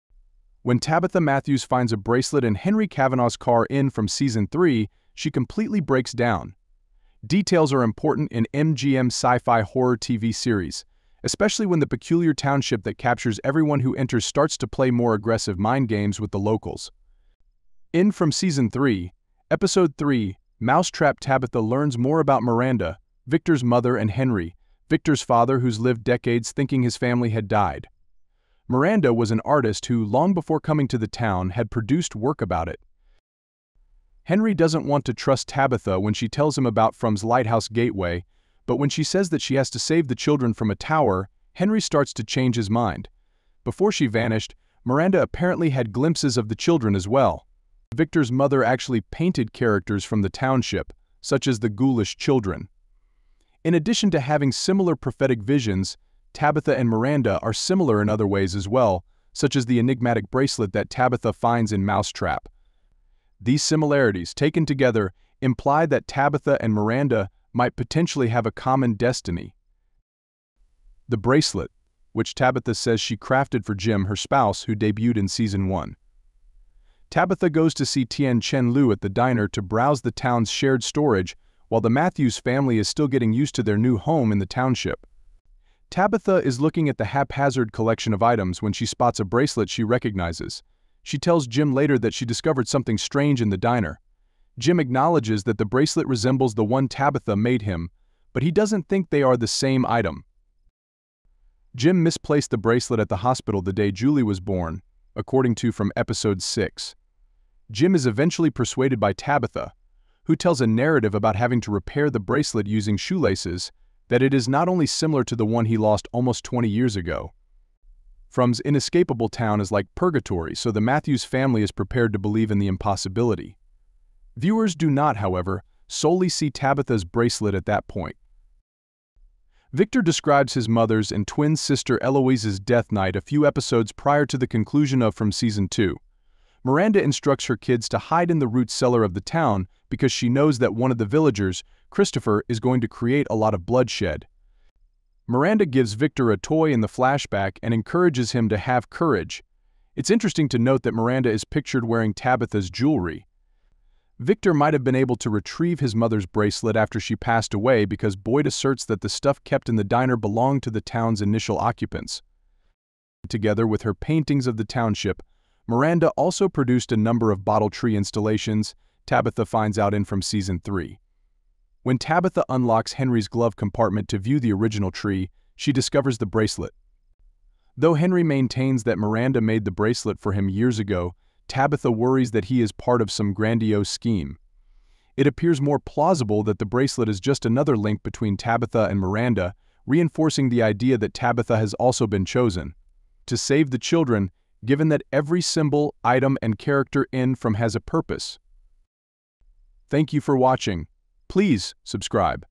FROM Season 3 : The Backstory Of The Bracelet Tabitha Finds In Episode 3
Our channel is about Movies Reviews. We cover lots of cool stuff such as Recap, Ending Explained and Breakdowns